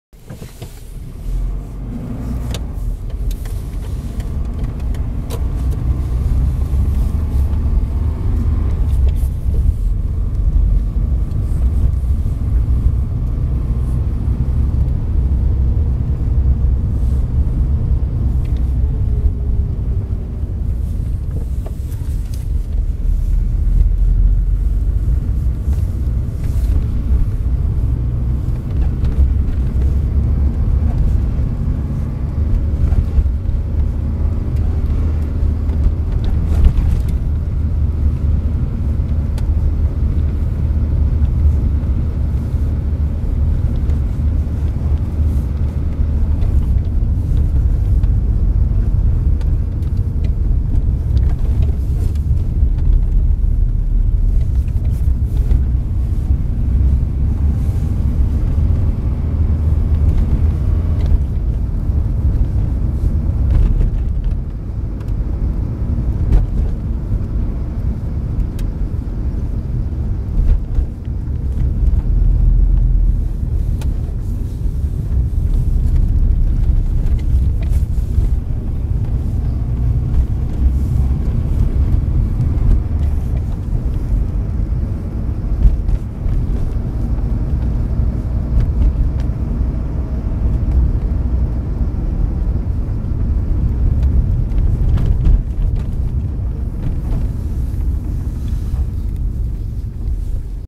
Biltur runt Coop Forum Lillån Örebro (2013)
Biltur runt det nu nedlagda Coop Forum i Lillån i Örebro, det som en gång i tiden hette Wessels och senare B&W Stormarknad. Numera finns XL Bygg, Dollarstore och MAX på en del av det som tidigare var parkeringsyta.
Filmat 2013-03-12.